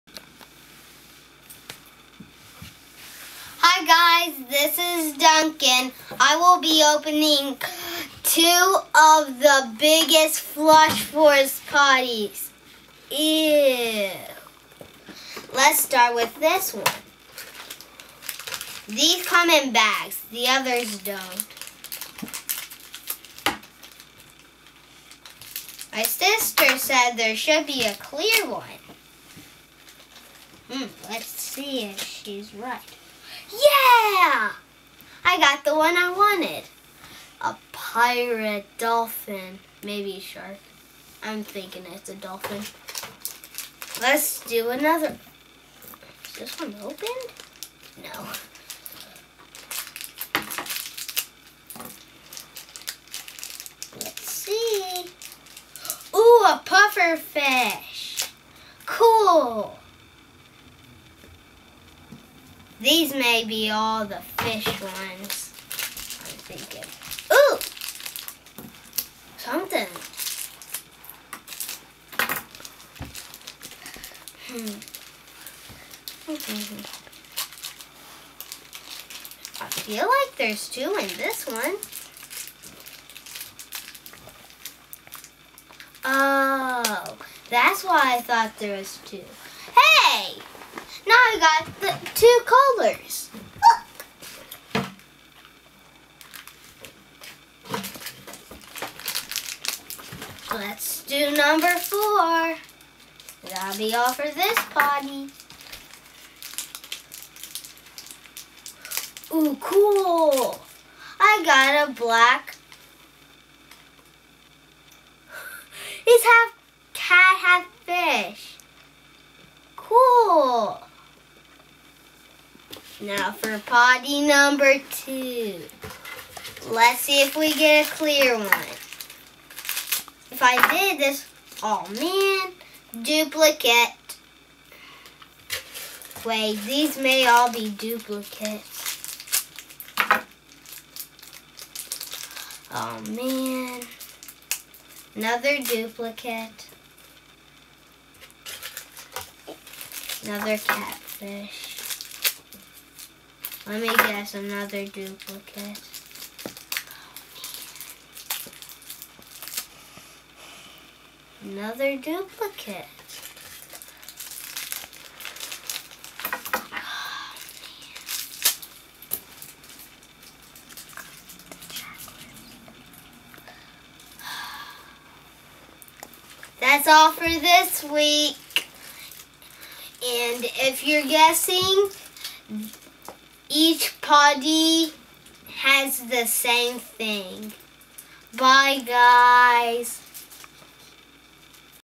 Duncan Opens 2 Flush Force Toilets
Duncan received 2 large Flush Force toilets for his birthday and he opens them both for this video.